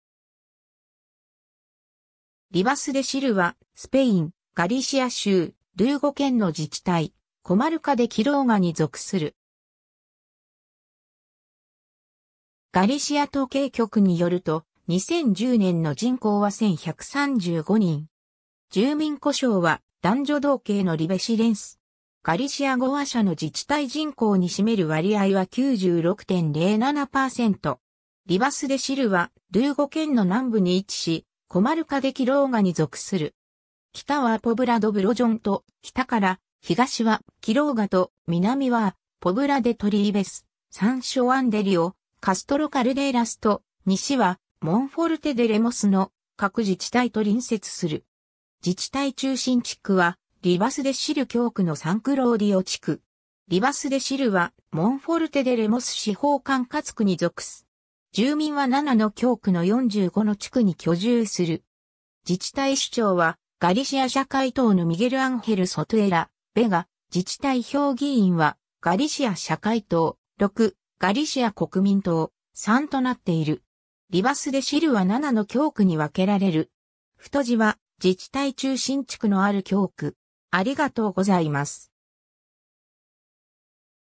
リバス・デ・シル
リバス・デ・シル（Ribas de Sil）はスペイン、ガリシア州、ルーゴ県の自治体、コマルカ・デ・キローガに属する。
ガリシア統計局によると、2010年の人口は1,135人（2009年：1,144人、2006年：1,245人、2005年：1,310人、2004年：1,335人、2003年：1,410人）。
住民呼称は、男女同形のribasilense。
 リバス・デ・シルはルーゴ県の南部に位置し、コマルカ・デ・キローガに属する。
北はア・ポブラ・ド・ブロジョンと、北から東はキローガと、南はア・ポブラ・デ・トリーベス、サン・ショアン・デ・リオ、カストロ・カルデーラス（3自治体ともオウレンセ県）と、西はモンフォルテ・デ・レモスの各自治体と隣接する。
自治体中心地区はリバス・デ・シル教区のサン・クローディオ地区。
 リバス・デ・シルはモンフォルテ・デ・レモス司法管轄区に属す。
 自治体首長はガリシア社会党（PSdeG-PSOE）のミゲル・アンヘル・ソトゥエーラ・ベガ（Miguel Angel Sotuela Vega）、自治体評議員はガリシア社会党：6、ガリシア国民党（PPdeG）：3となっている（2011年5月22日の自治体選挙結果、得票順）。
 リバス・デ・シルは7の教区に分けられる。